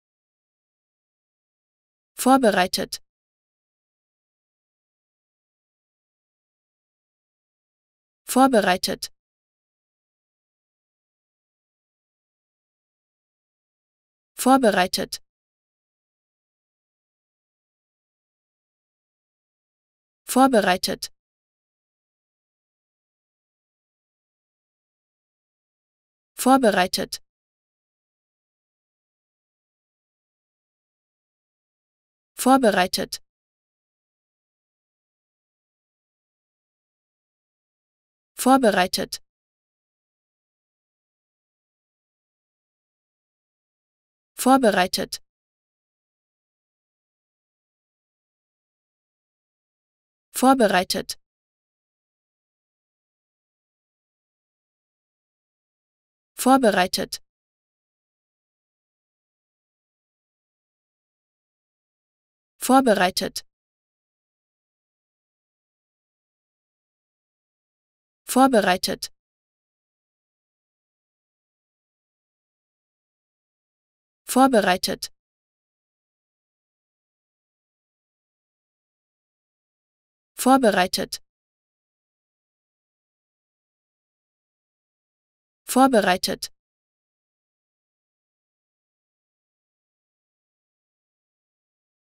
How to Pronounce ''vorbereitet'' (prepared) Correctly in German
Learn how to say ''vorbereitet'' in German with this short pronunciation tutorial.